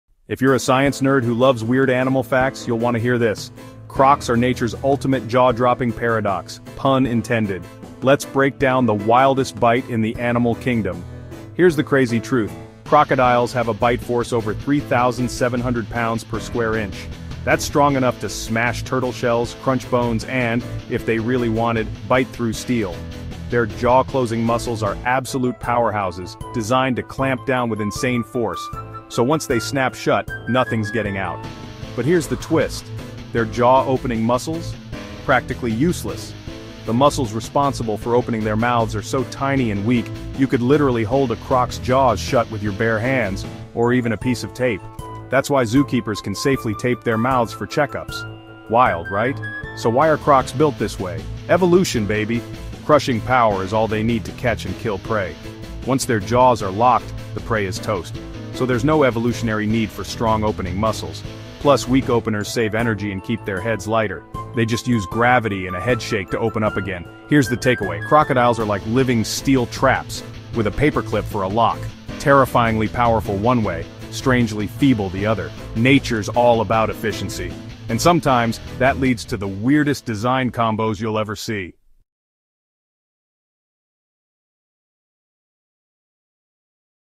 Crocodile Jaw Power Explained
Discover the jaw-dropping science behind crocodiles' insanely powerful bite force—over 3,700 psi! 🦷 Learn how these prehistoric predators crush bones and shells with hydraulic-like muscle strength, yet have surprisingly weak jaw-opening muscles. Why does evolution give them such a deadly clamp but only feeble opening power? It’s all about energy efficiency, safety, and survival strategy! Find out how a human hand or even a rubber band can keep their jaws shut, making crocs both terrifying and oddly vulnerable. Perfect for science fans curious about animal biology and evolutionary quirks.